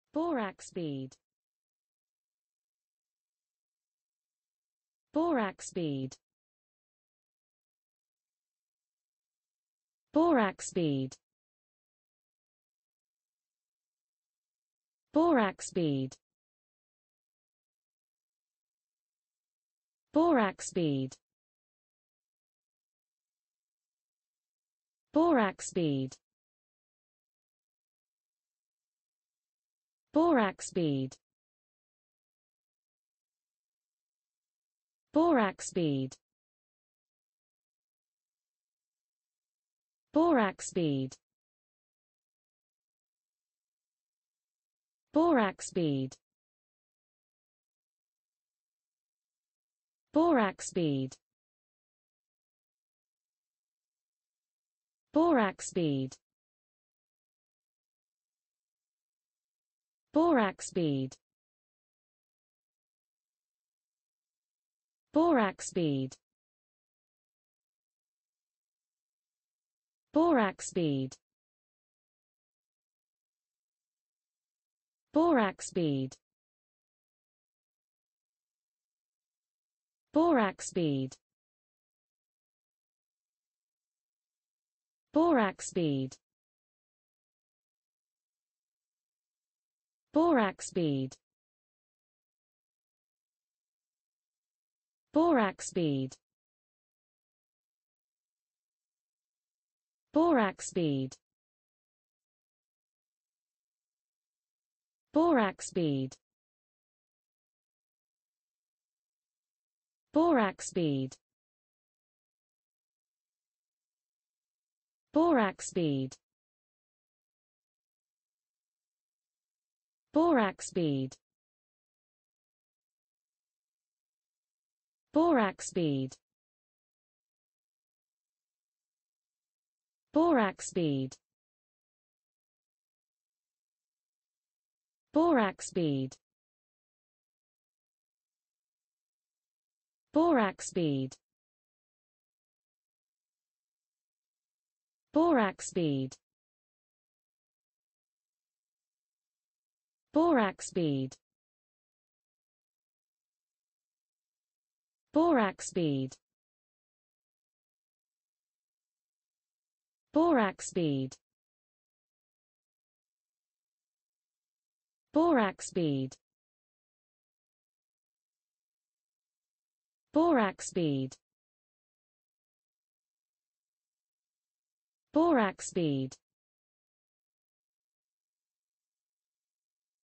Borax Bead: How to pronounce Borax Bead in english (correct!).Start with B. Learn from me.
Follow me!Learn from me how to pronounce the word Borax Bead in english.This video shows you how to pronounce the word Borax Bead in english.

null